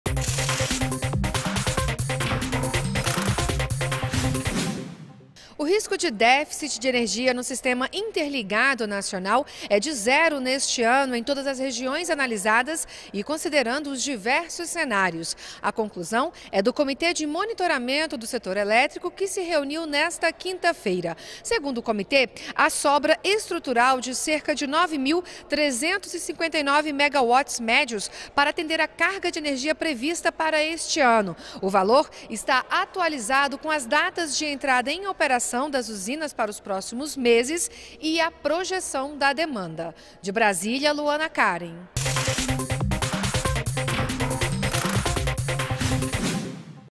Brasil tem risco zero de déficit de energia
ÚLTIMAS - 09.10.15: O Comitê de Monitoramento do Setor Elétrico se reuniu nesta quinta-feira (8) e previu risco zero de déficit de energia no Sistema Interligado Nacional. Segundo o comitê, a sobra estrutural para atender a carga de energia prevista para este ano está atualizada com as datas de entrada em operação das usinas e a projeção da demanda.